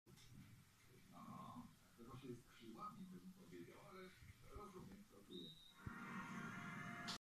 Ćwiczenie 1 przy plecach płaskich
Pozycja wyjściowa: leżenie na plecach, ręce wzdłuż tułowia, nogi ugięte w kolanach i biodrach
Ruch: "rowerek"